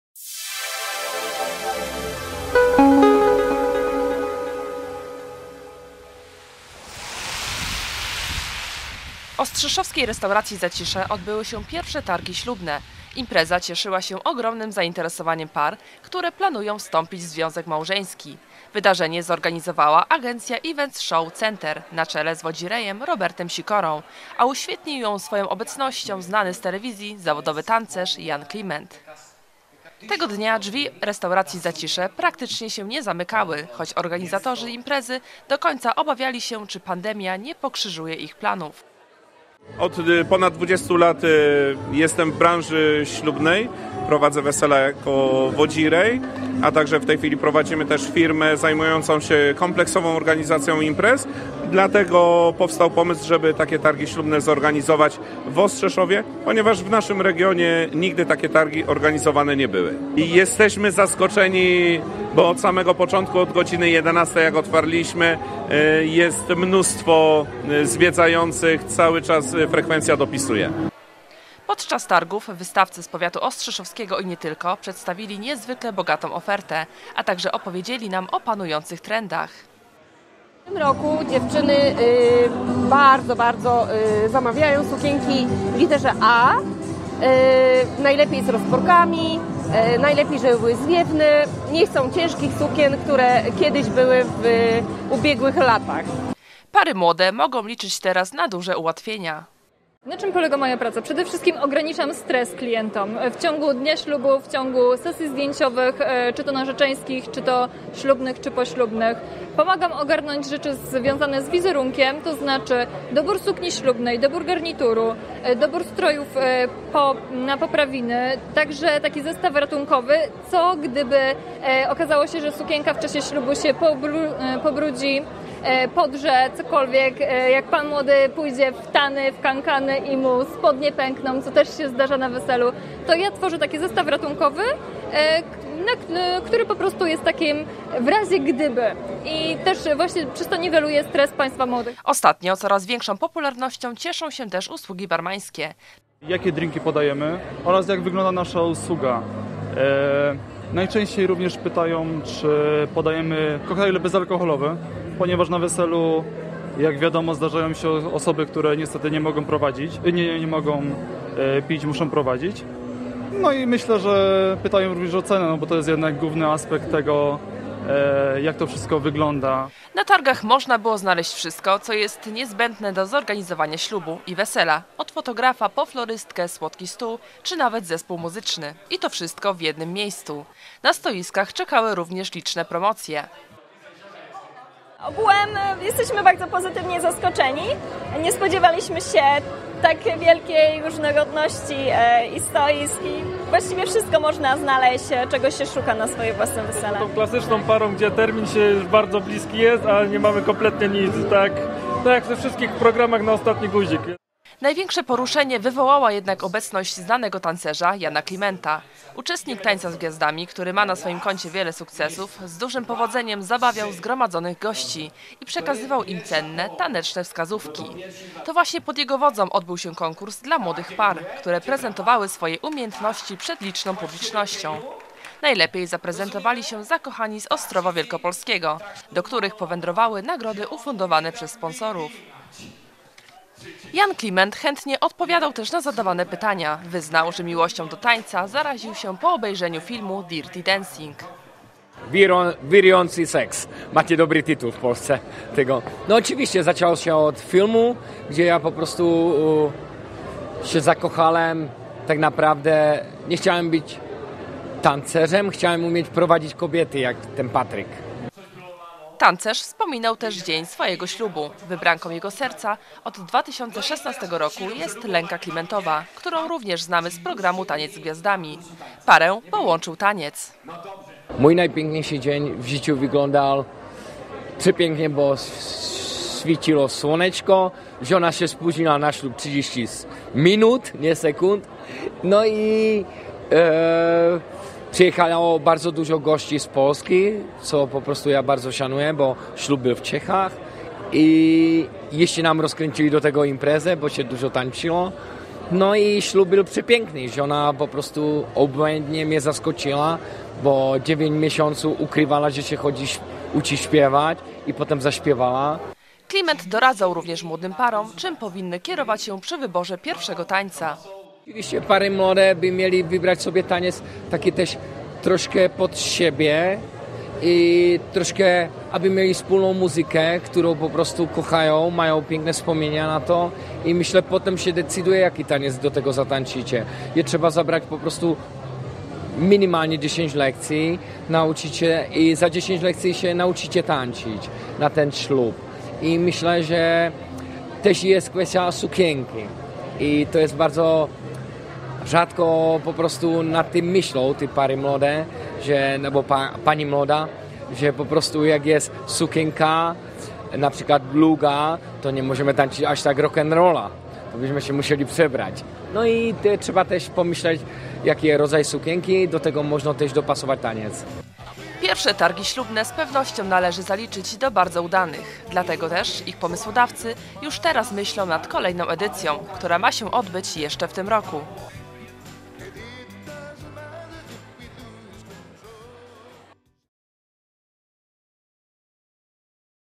Targi Ślubne z udziałem Jana Klimenta zgromadziły tłumy
W ostrzeszowskiej restauracji "Zacisze" odbyły się pierwsze Targi Ślubne. Impreza cieszyła się ogromnym zainteresowaniem par, które planują wstąpić w związek małżeński. Wydarzenie zorganizowała Agencja Events Show Center na czele z wodzirejem Robertem Sikorą, a uświetnił ją swoją obecnością znany z telewizji zawodowy tancerz Jan Kliment.